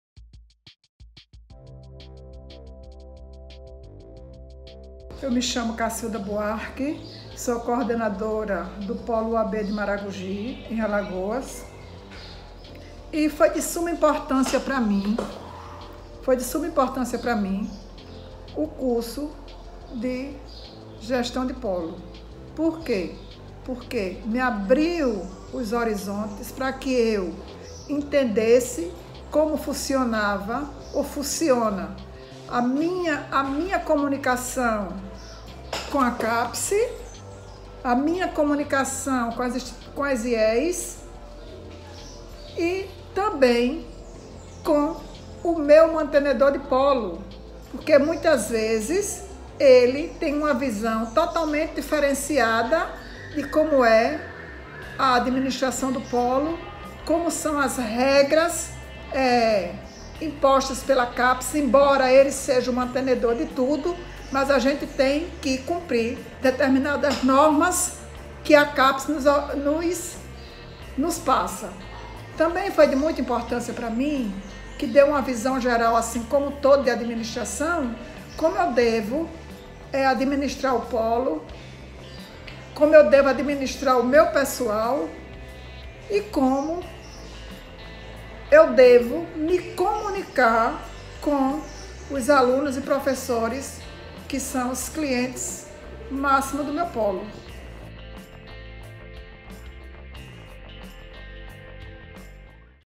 [DEPOIMENTOS] Gestão de Polos - Maragogi (AL)
Depoimento diretamente de Alagoas, de Cacilda Buarque Silva (Polo UAB de Maragogi- AL) sobre o curso de aperfeiçoamento em Gestão Articulada de Polos UAB. O curso é possível graças a uma parceria da Universidade Estadual do Centro-Oeste do Paraná com a Capes e com o Fórum Nacional de Coordenadores de Polos UAB. Material desenvolvido pelo Nead Unicentro e hospedado no Licon, plataforma de Livre Conhecimento.